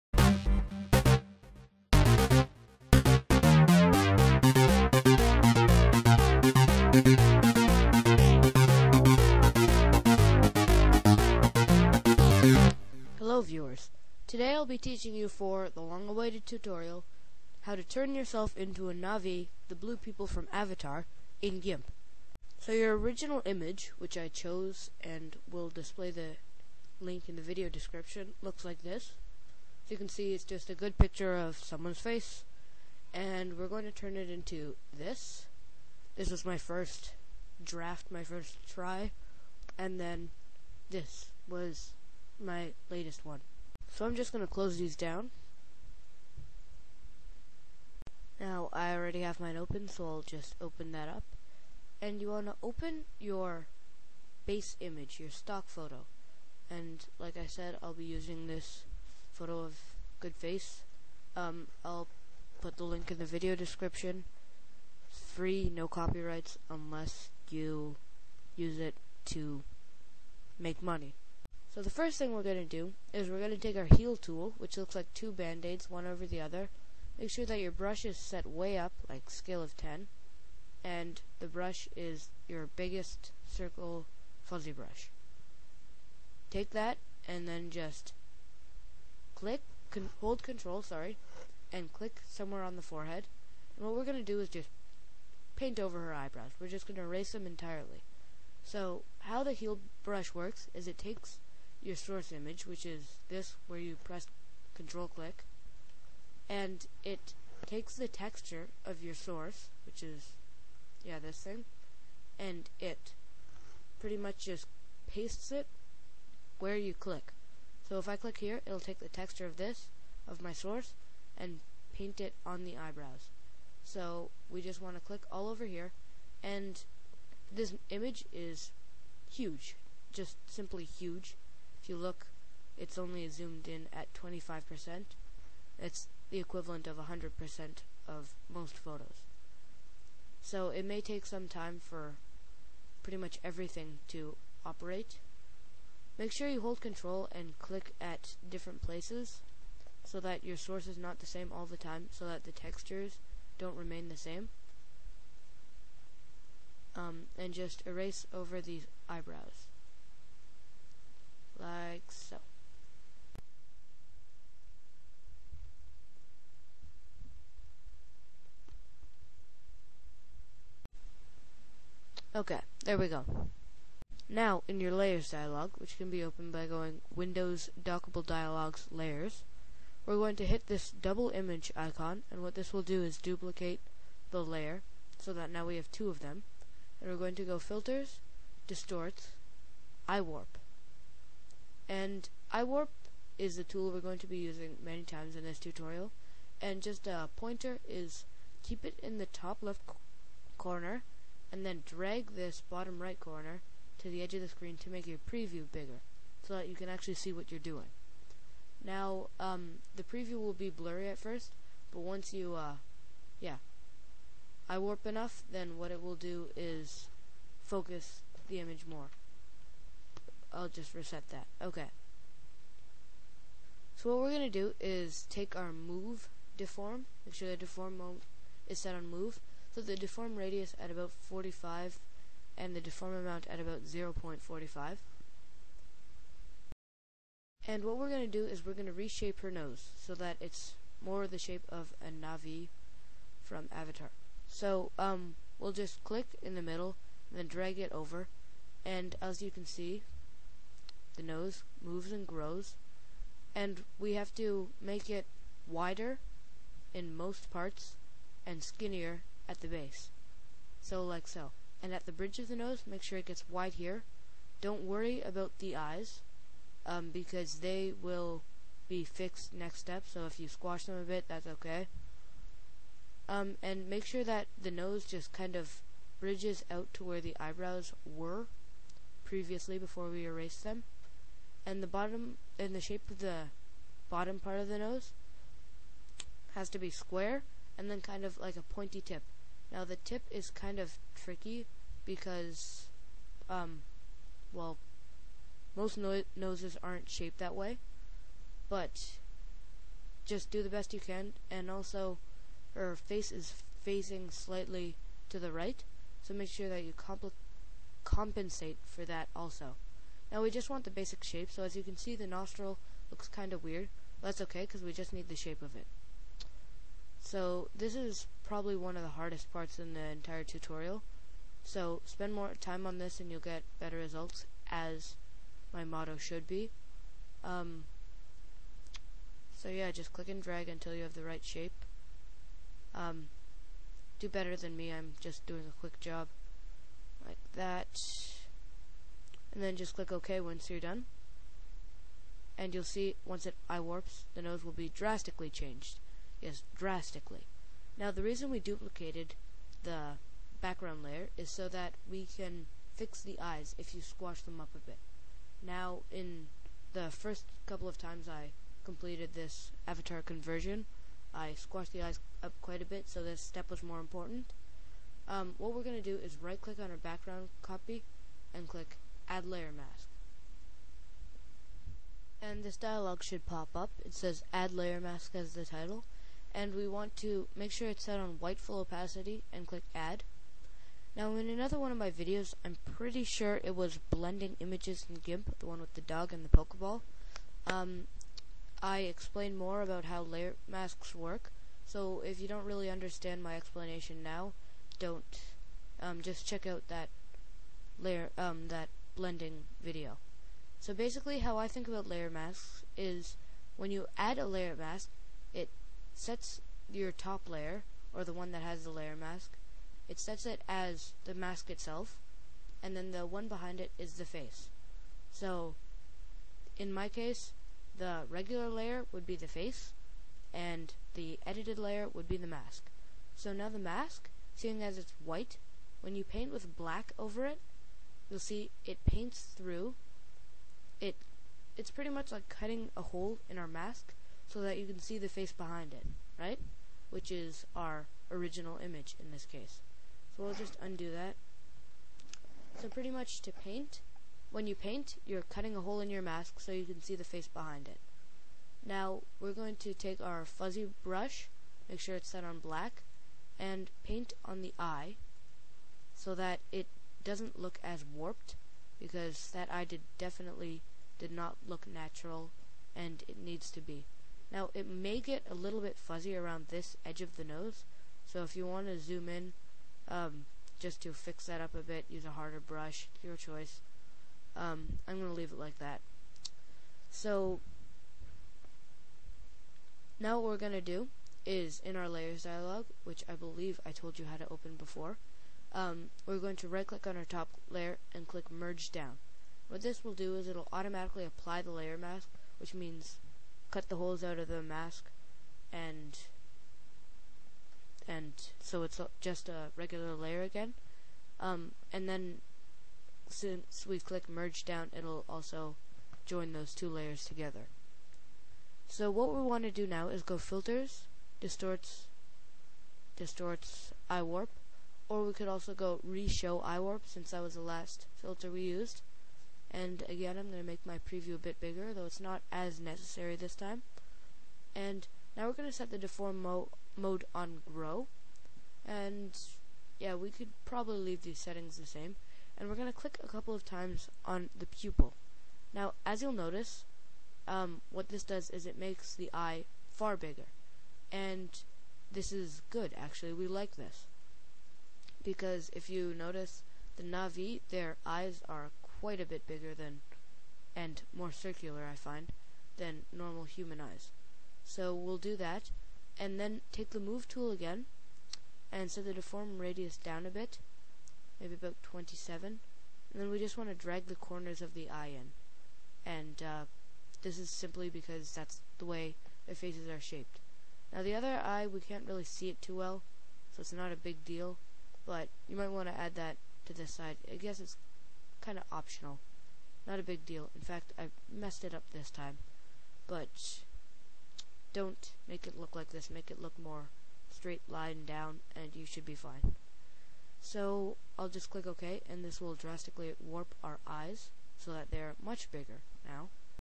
Changing into a Na'vi from Avatar in Gimp(Part 1)
Changing into a Na'vi from Avatar in Gimp

Learn how to convert a picture of yourself into one of the blue people from James Cameron's AVATAR in The Gimp. Gimp is a free alternative to Photo Shop.
I apologize for the length of this tutorial. My excuse is that I go into a LOT of detail.

Resources and links:
I found out how from much googling and intuition. Here is one of my main sources: a photoshop tutorial.

Difficulty Level:
7.5/10 = Advanced

Feel free to post tutorial suggestions in the comments section of my channel.